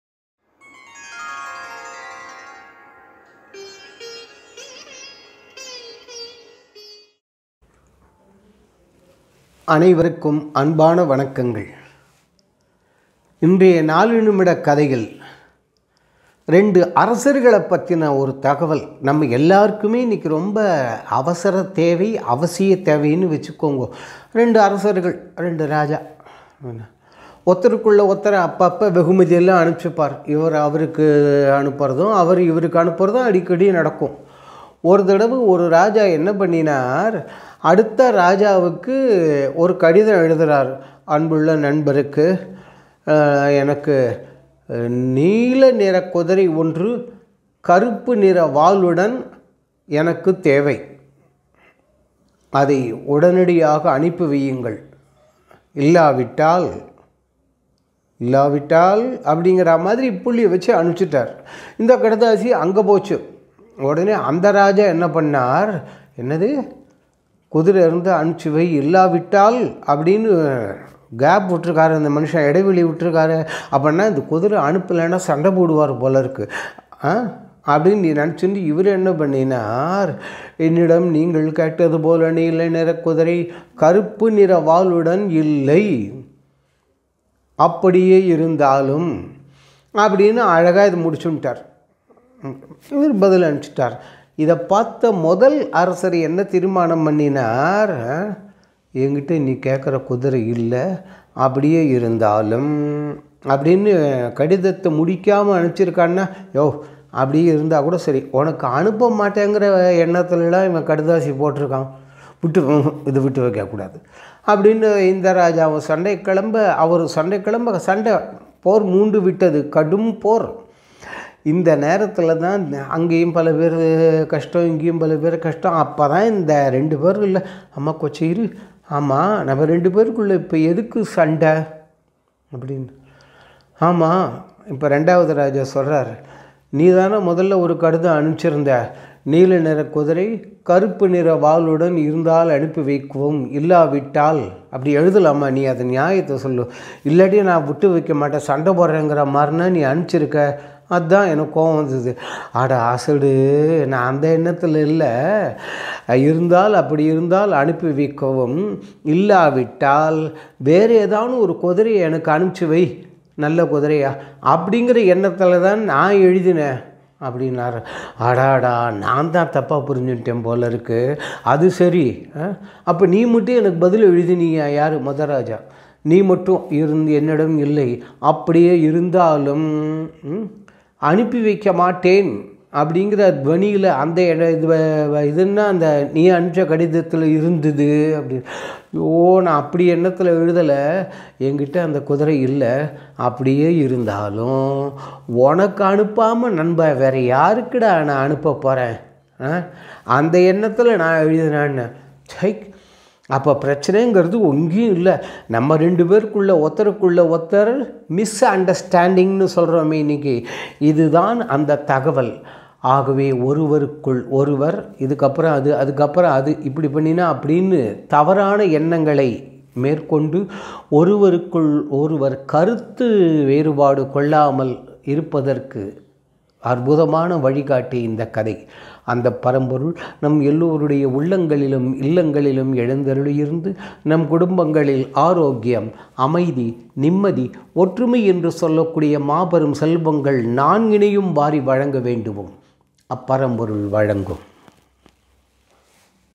இரண்டு அரசர்கள் | நான்கு நிமிடக் கதைகள்
இரண்டு அரசர்கள் நண்பர்களாக இருந்தனர்.  ஒருமுறை அவர்களுக்குள் நடந்த ஒரு தகவல் பரிமாற்றத்தில் ஏற்பட்ட குளறுபடியால் என்ன விளைந்தது என்பதையும், அதன் மூலம் நமக்குத் தெரிய வருவது என்ன என்பதையும்...  வாருங்கள், காணலாம்!